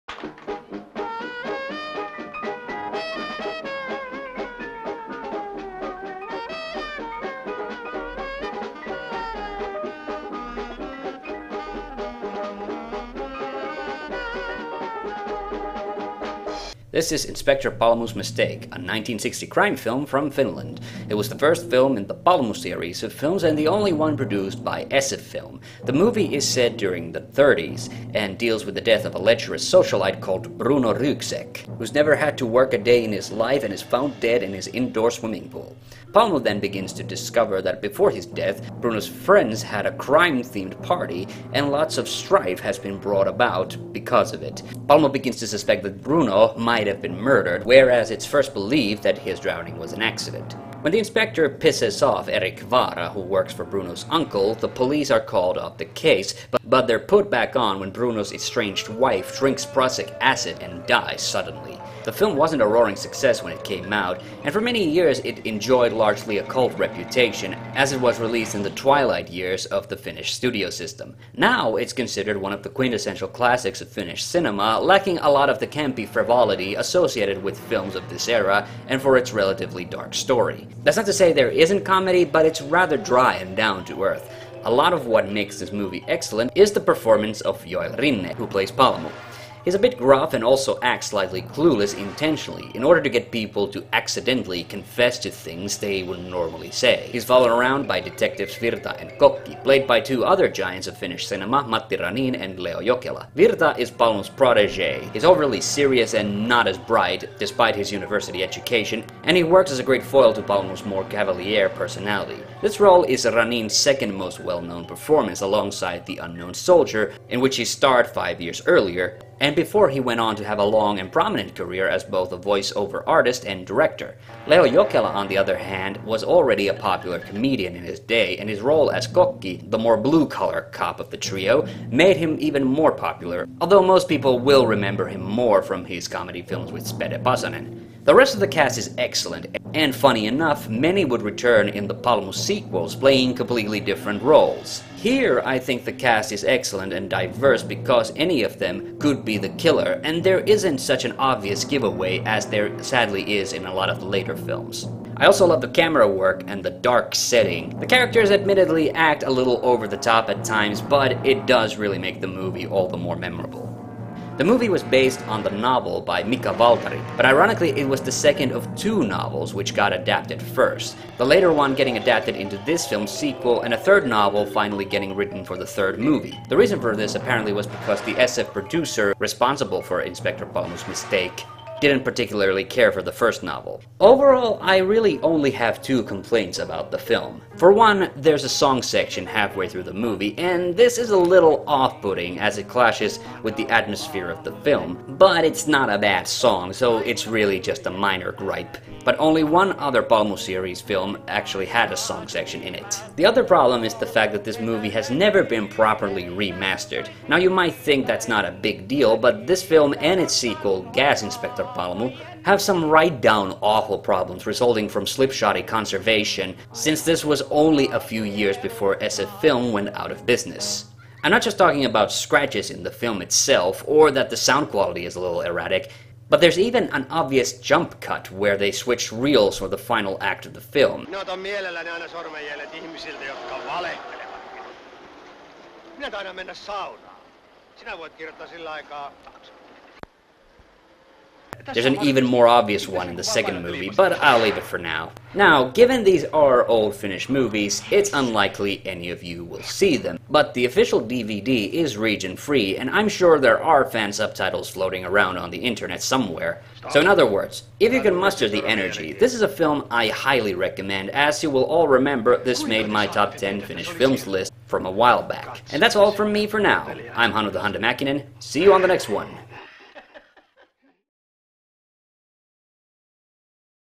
TheHande Reviews Inspector Palmu's Mistake
Reviewing a classic of Finnish cinema.